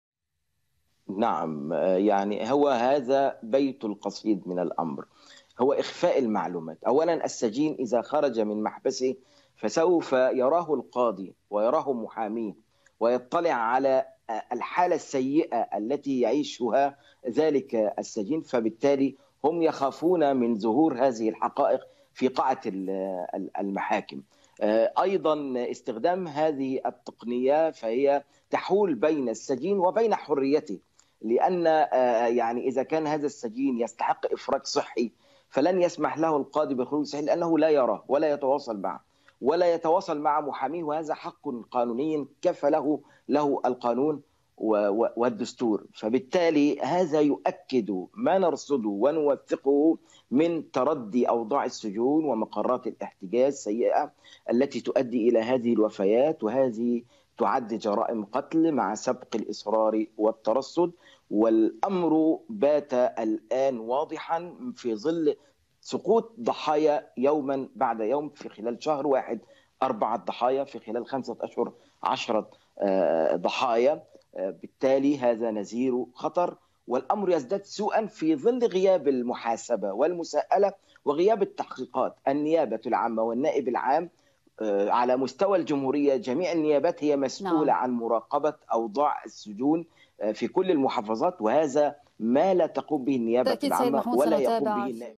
مدير مؤسسة عدالة: النظام يخشى من ظهور المعتقلين بقاعة المحاكم نظرا للظروف الصعبة التي يعانون منها
المحامي والحقوقي "محمود جابر" مدير مؤسسة عدالة لحقوق الإنسان: النظام يخشى من ظهور المعتقلين في قاعة المحاكم نظرا للظروف الصعبة التي يعانون منها داخل